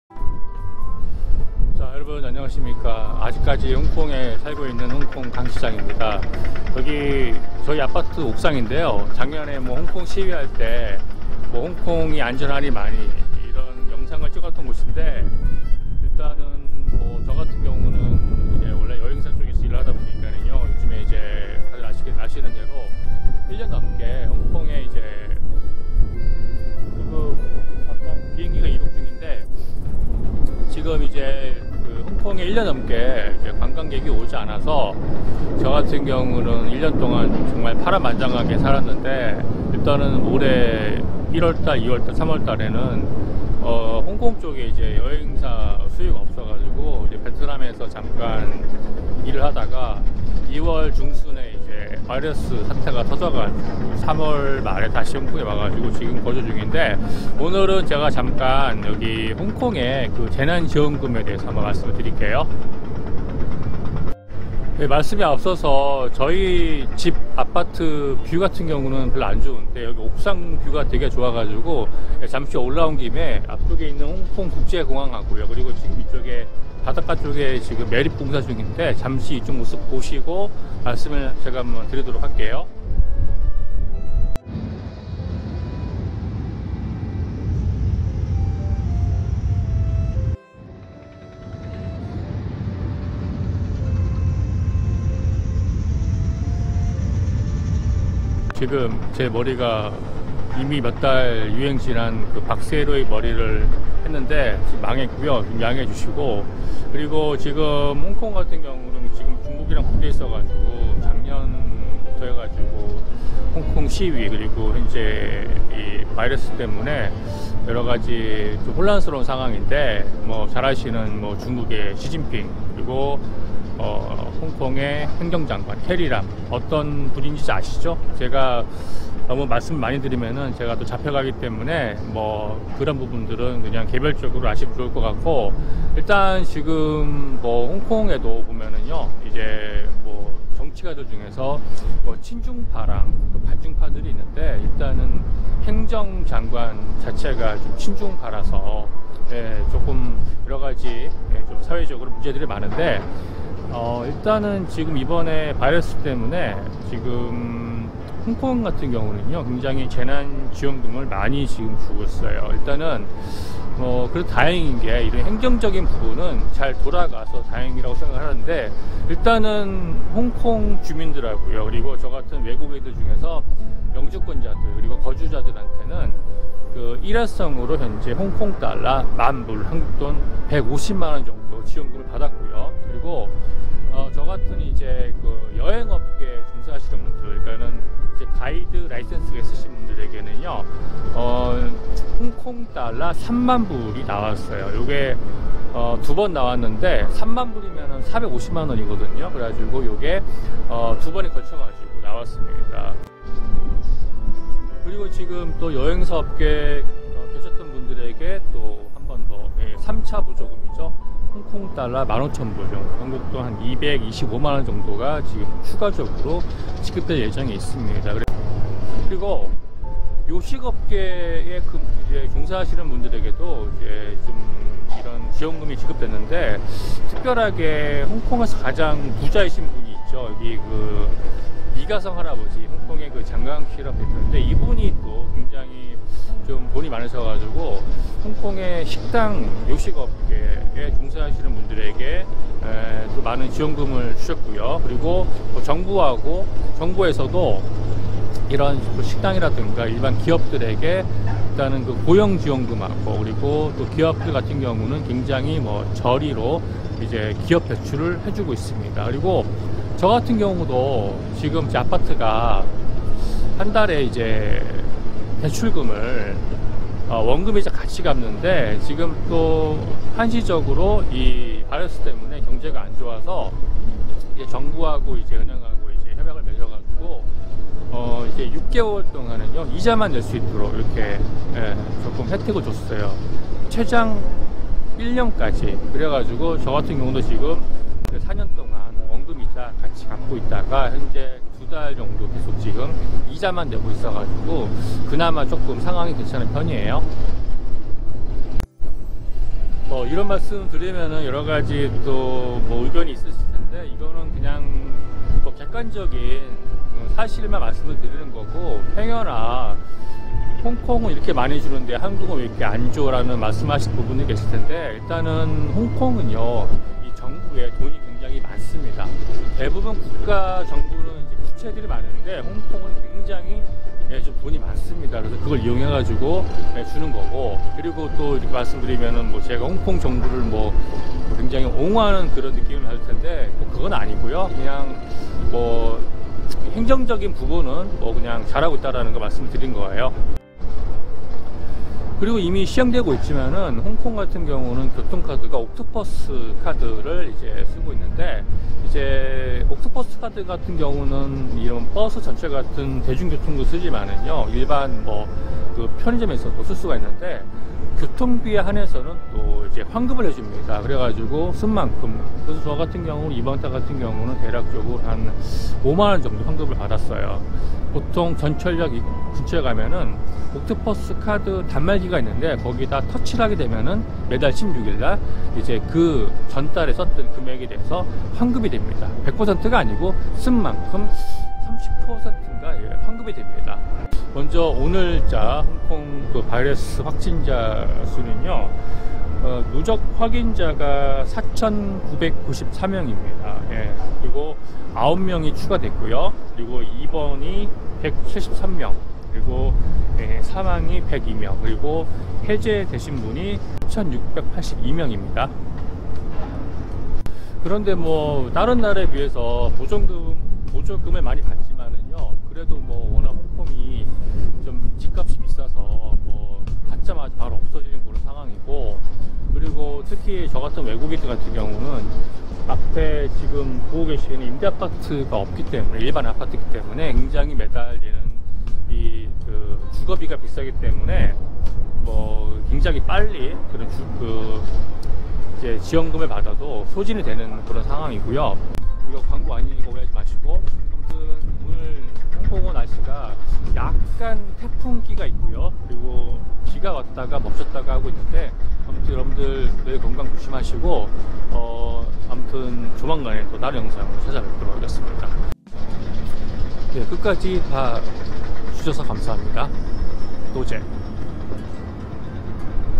역대 최고 바이러스 홍콩 긴급 재난 지원금
바이러스로 많은 피해를 받아 홍콩 정부에서 집행한 홍콩 재난 지원금 현황입니다.

안녕하세요!!!